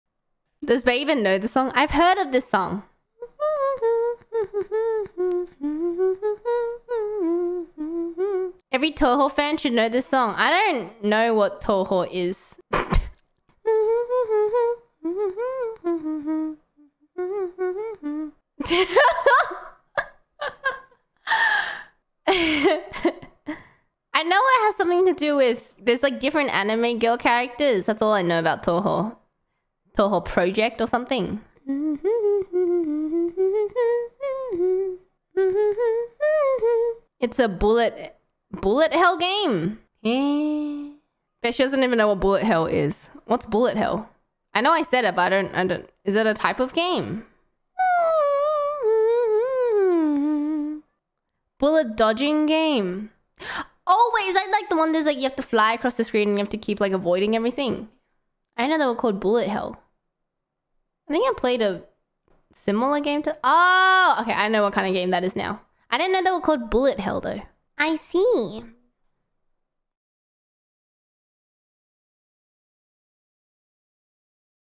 【悲報】オタクのハコ太郎、東方プロジェクトを知らないせいで視聴者にがっかりされてしまう...【日英字幕/切り抜き/ホロライブ/ハコス・ベールズ】
▼元動画